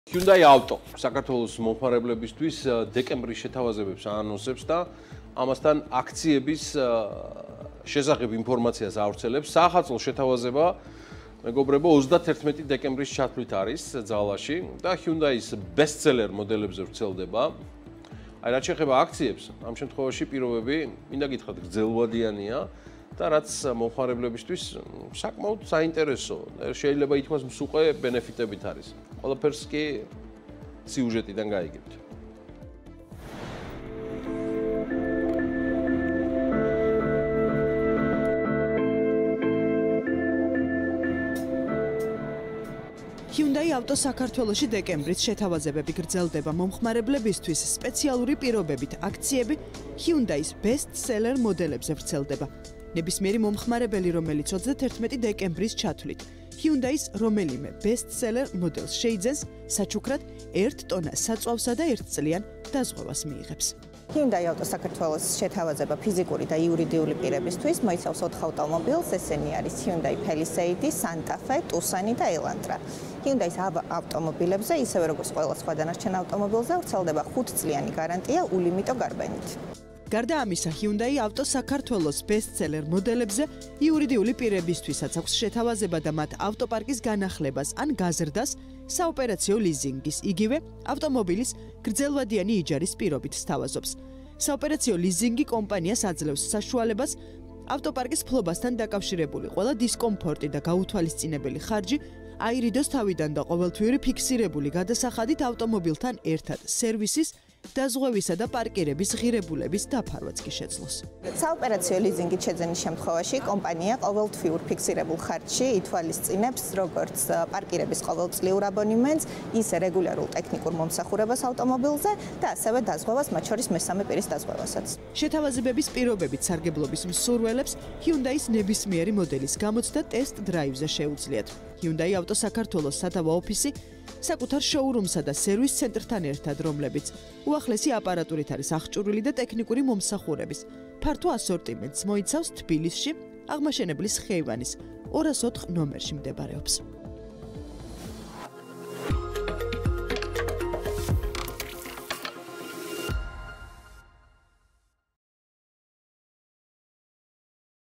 Hyundai Auto Georgia სადეკემბრო შეთავაზებები
ჰიუნდაი ავტო საქართველო მომხმარებლებისთვის დეკემბრის შეთავაზებებს აანონსებს და ამასთან აქციების შესახებ ინფორმაციას ავრცელებს. საახალწლო შეთავაზება 31 დეკემბრის ჩათვლით გრძელდება და ჰიუნდაის ბესტსელერ მოდელებზე ვრცელდება, რაც შეეხება აქციებს, ამ შემთხვევაში პირობები გრძელვადიანია და რაც მომხმარებლებისთვის საკმაოდ მსუყე ბენეფიტებით.